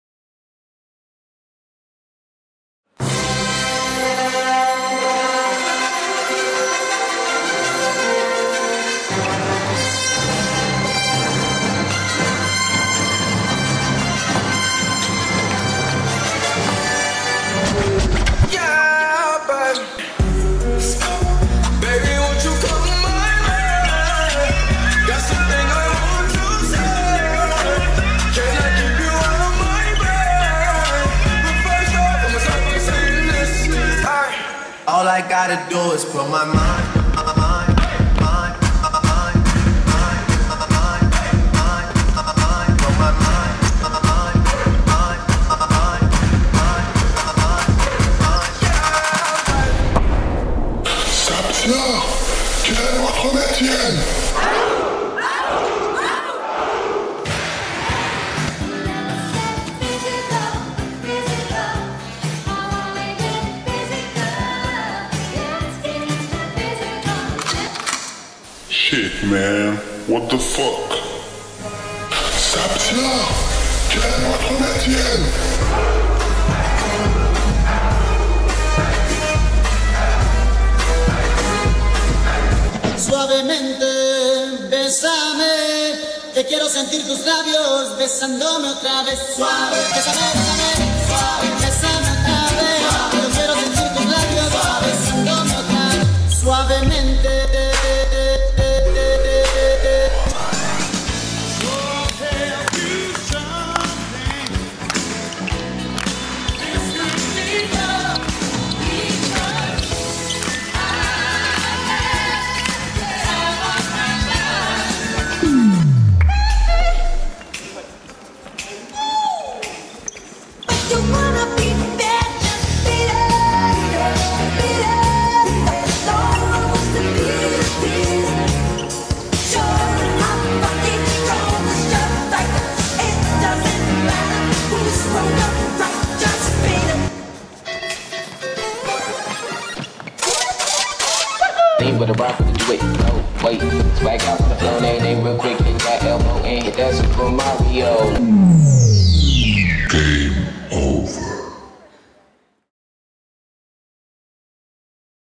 Rouen Normandie Sup' Cup 2016 : la choré STAPS
La Sup’ Cup réunira les étudiants de Rouen les 17 et 19 novembre. Au programme, volley, balle aux prisonniers, parcours aquatique, mölkky… Mais l'événement sportif et festif débute dès maintenant avec le concours de chorégraphies. Découvrez les prestations de chaque établissement et partagez votre vidéo préférée pour faire gagner votre équipe !

Réalisation : Direction de l’information et de la communication externe - Métropole Rouen Normandie